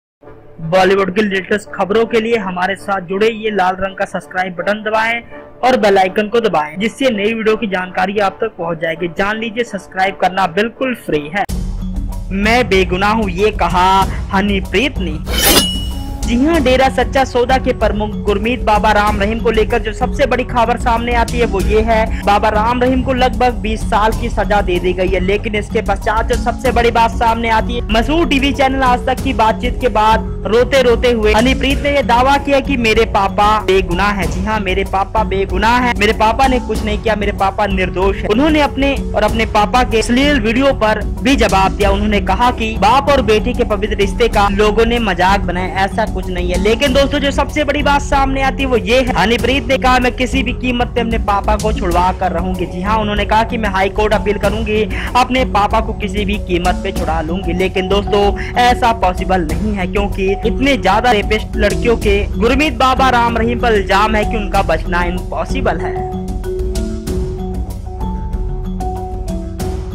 मैं किसी भी कीमत पर पापा को बचाऊंगी पापा गलत नहीं है। Honey Preet Interview Jail
मैं किसी भी कीमत पर पापा को बचाऊंगी पापा गलत नहीं है। Honey Preet Interview Jail News Hollywood News All type news hindi and English